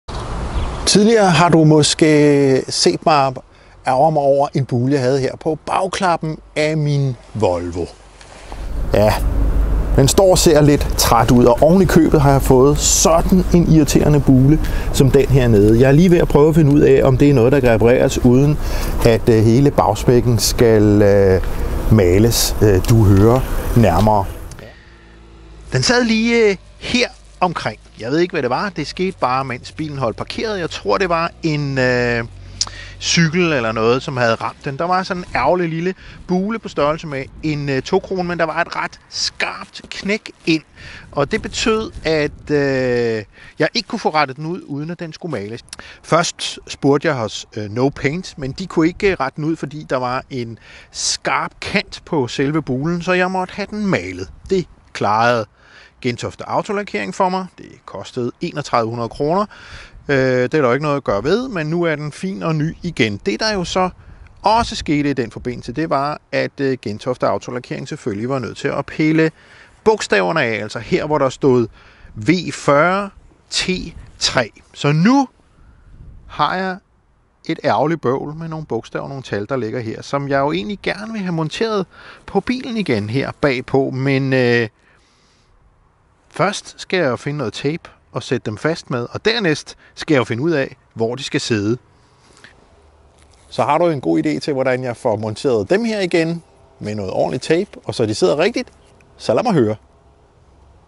Så' der bøvl med chefens Volvo IGEN!
Chefredaktør på Bil Magasinet, Mikkel Thomsager får en ærgerlig lille bule på bagsmækken af sin Volvo V40. Bilen bliver malet, men hvad gør han med de bogstaver, der sad på bagsmække før? Se med i denne video, hvor vi igen følger livet med redaktionens egne køretøjer.